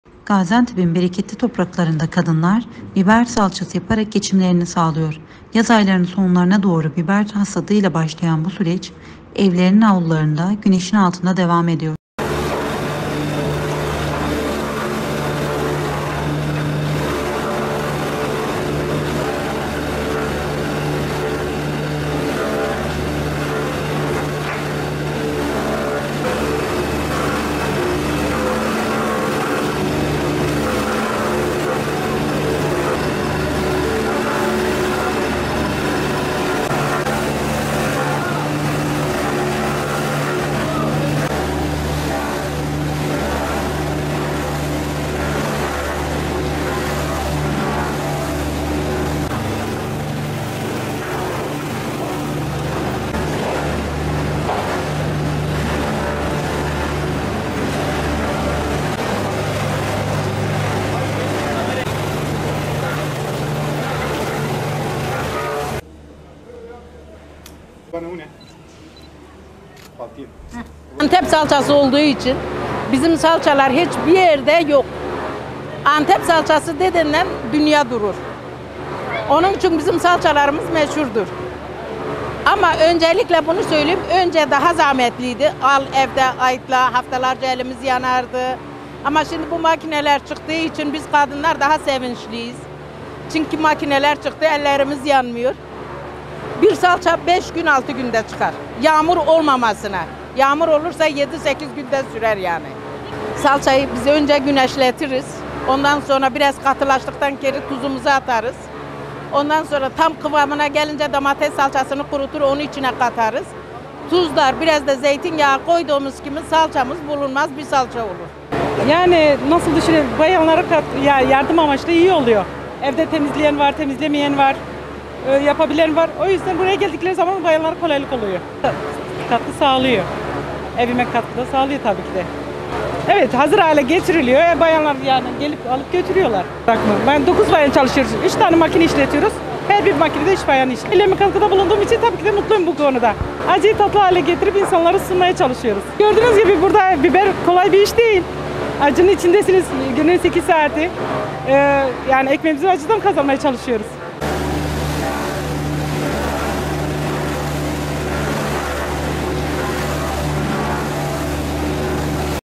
Kadınlar Ekmeklerini 'Acı'dan Çıkarıyor
Gaziantep'in bereketli topraklarında kadınlar, biber salçası yaparak geçimlerini sağlıyor. Yaz aylarının sonlarına doğru biber hasadıyla başlayan bu süreç, evlerin avlularında, güneşin altında devam ediyor.